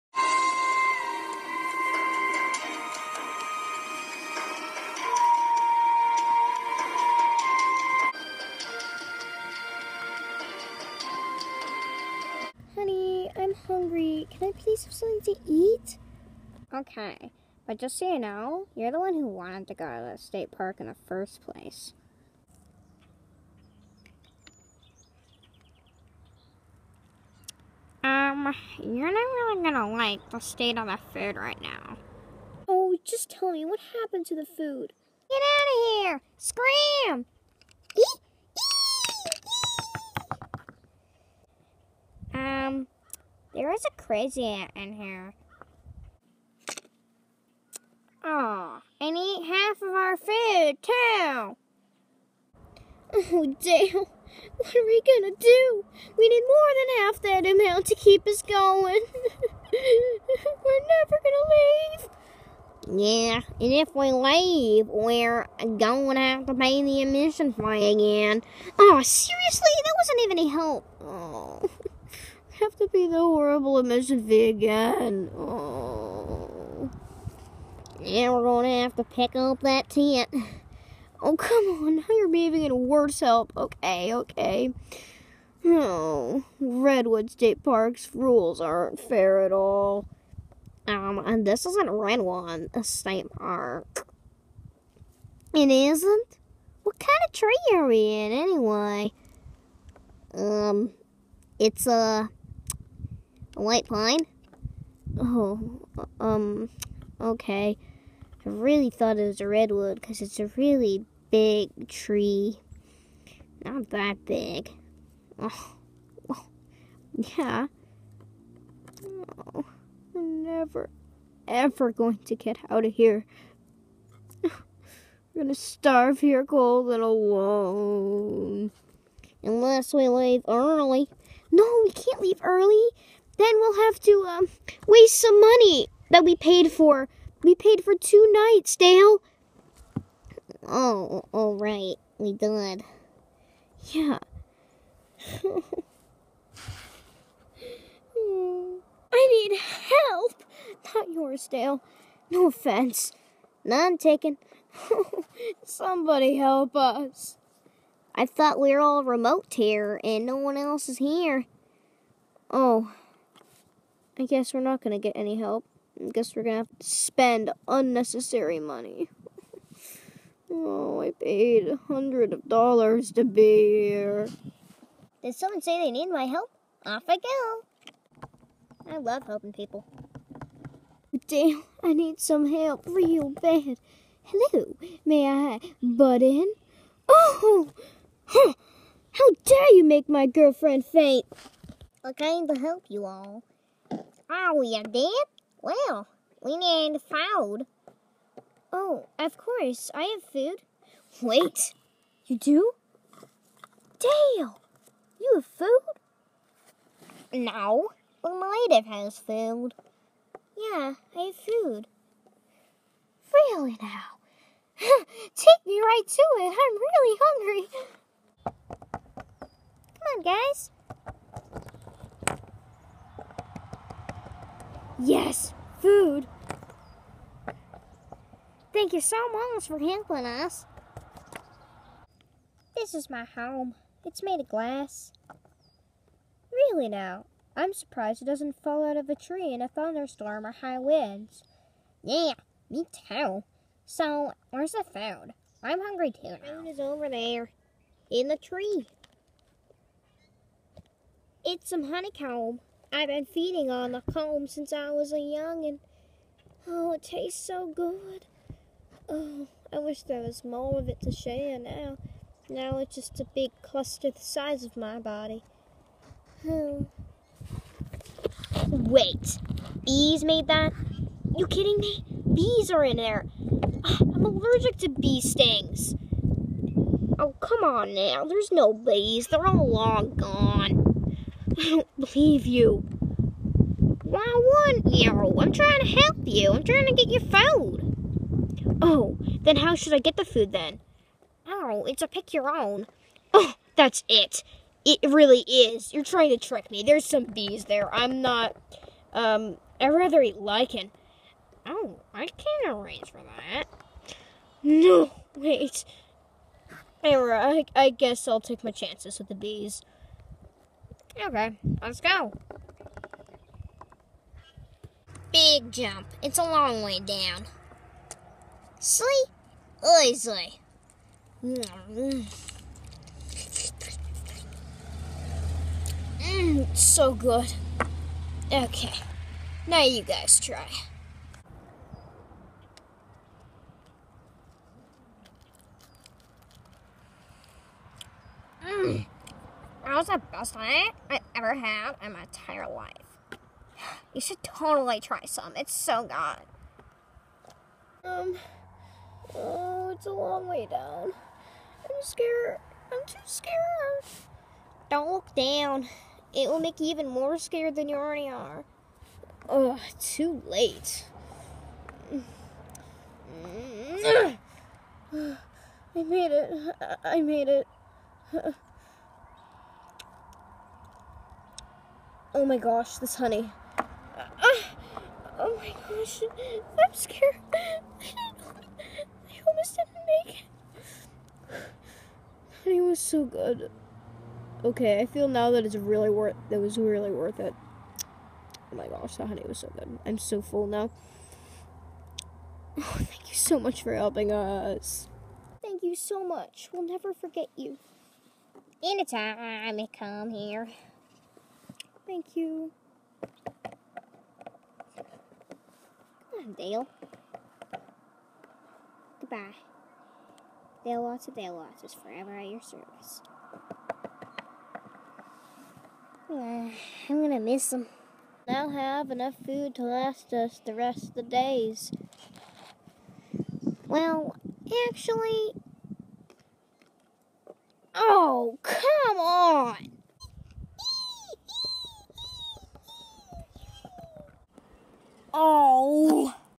LPS: The food shortage
When Cat and her boyfriend Dalewattsindalewatts (Dale) run out of food when they are camping, they have to trust a stranger to get more.